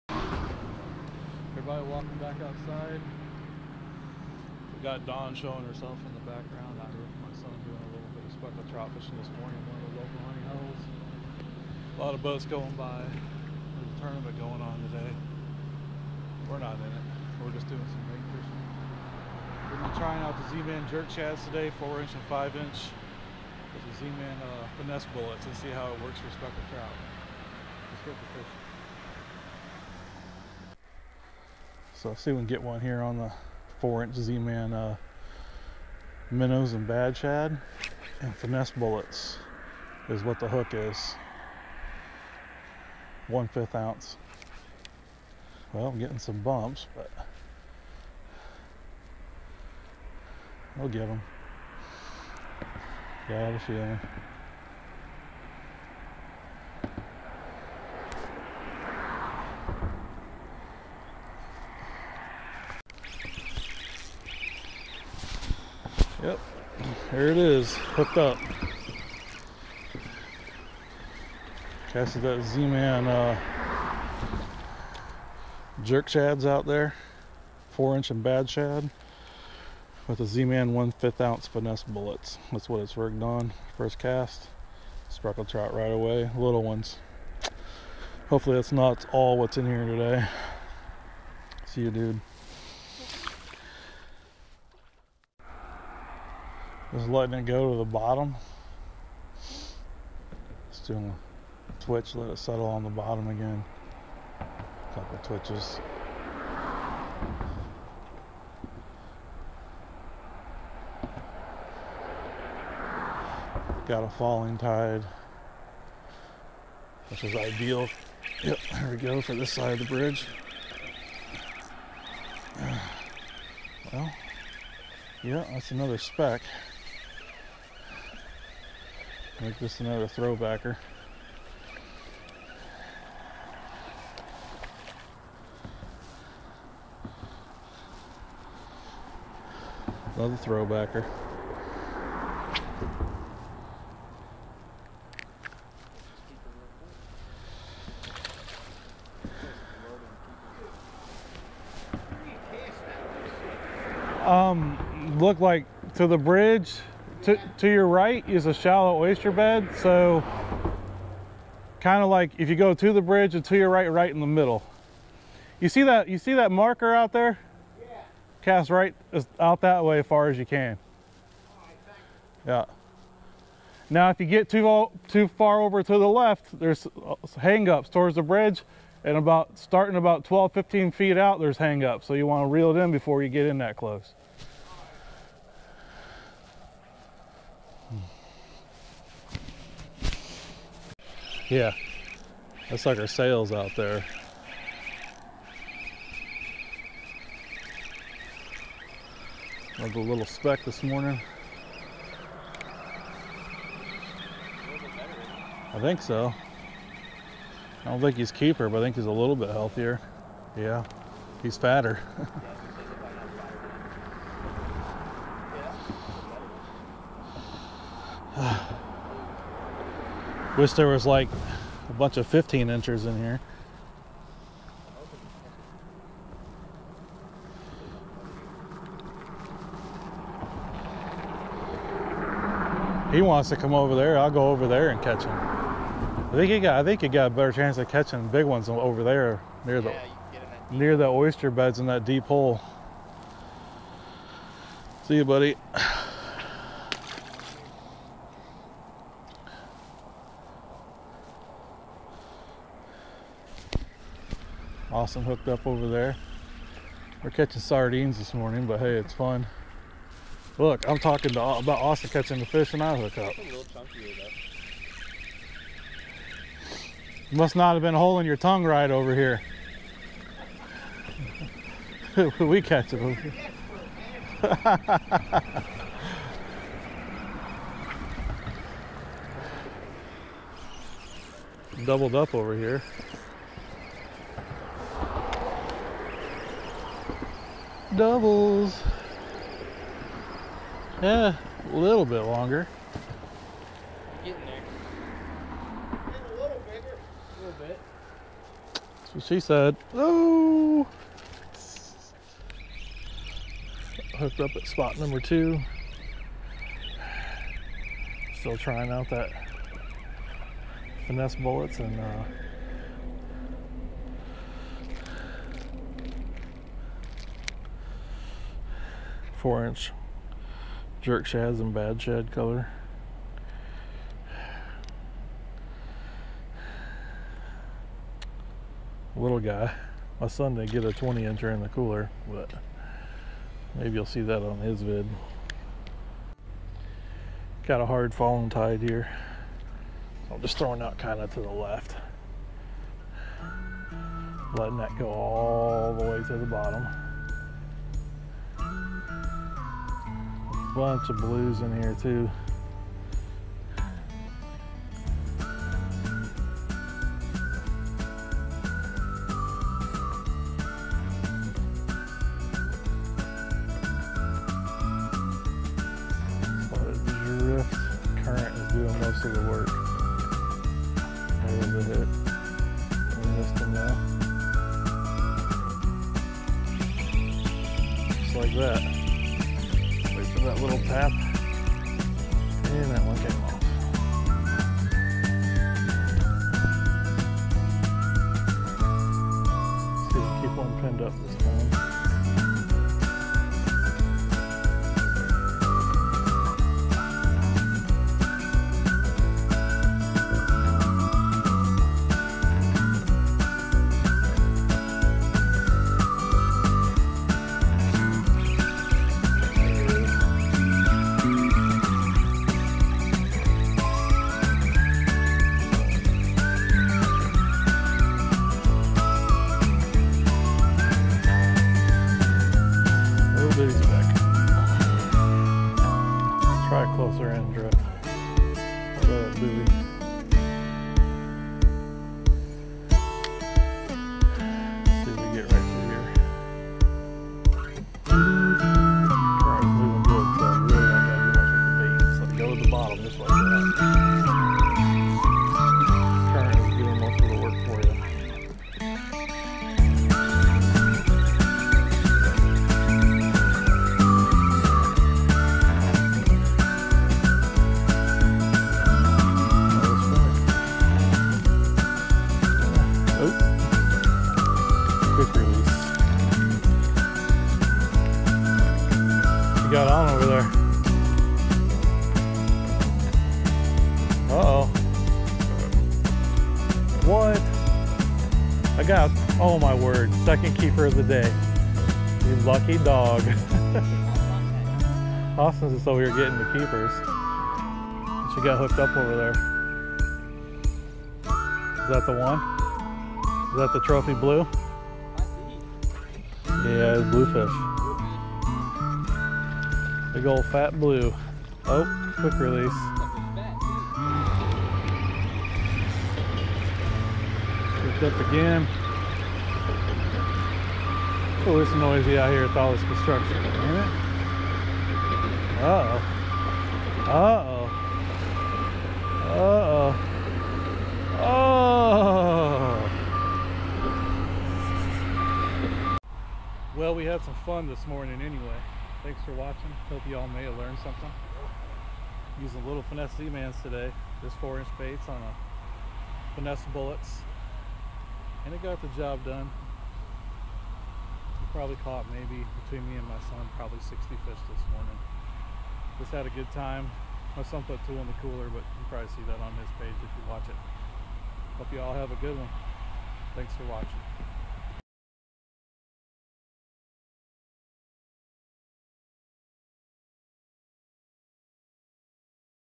Trying Z-Man Finesse Bulletz in Saltwater
Went out with my son Austin on early morning trip targeting speckled trout with Z-Man Finesse baits. mainly used the Bulletz and 4 inch Jerk Shadz to what I could catch. Z-Man